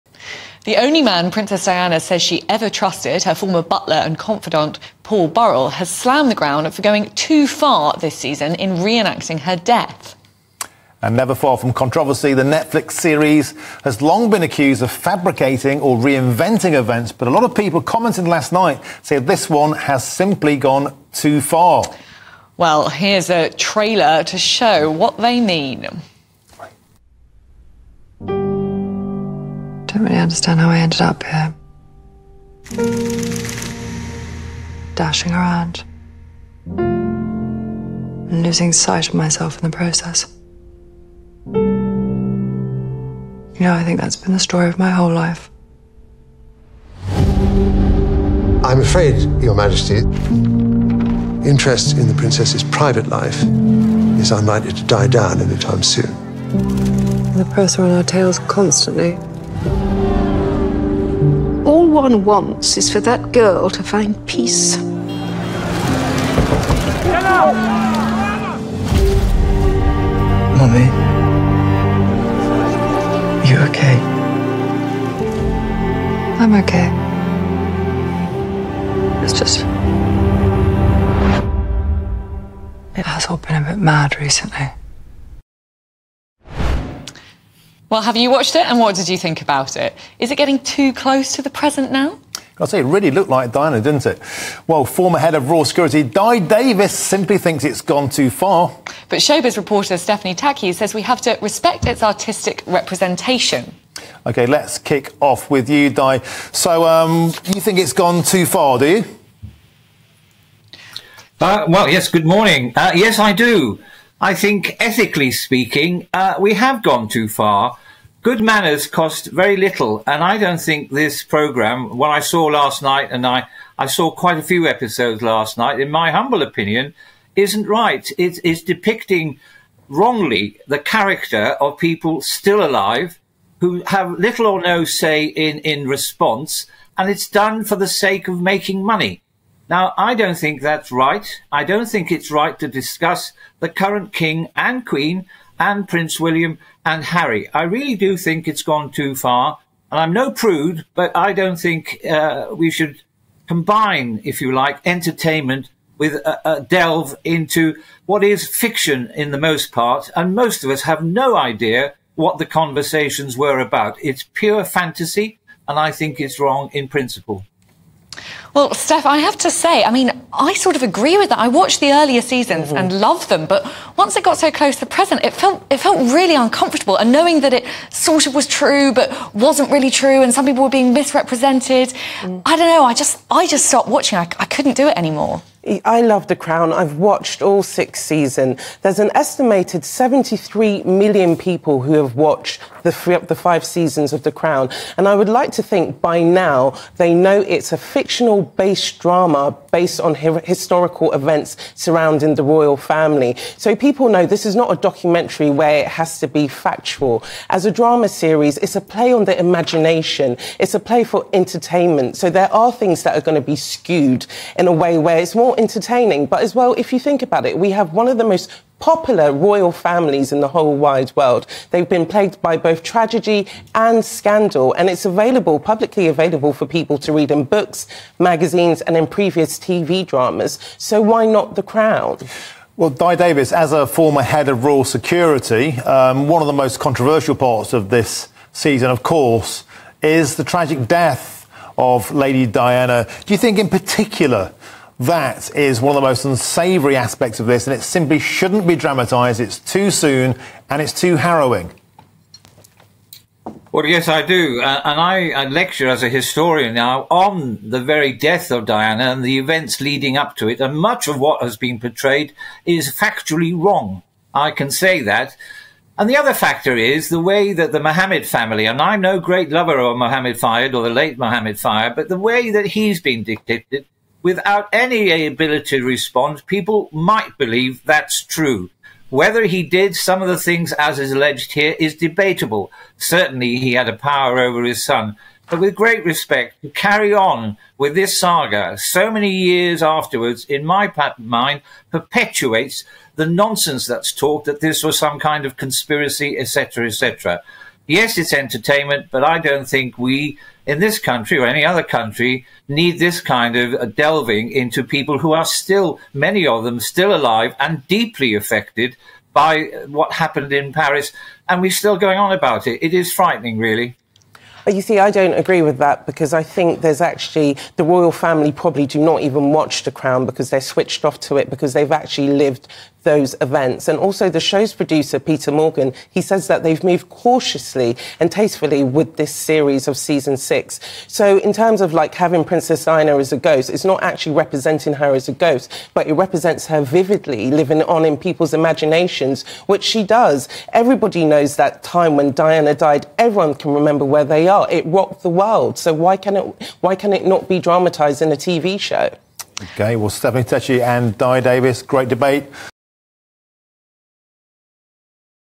The Crown 'has GONE TOO FAR': Dai Davies accuses the series of 'making money' from 'pure fantasy'
Former Head of RoyaL Security, Dai Davies and Showbiz Reporter and Journalist, Stephanie Takyi, debate if the latest series of The Crown on Netflix is an accurate and ethical depiction of real-life events.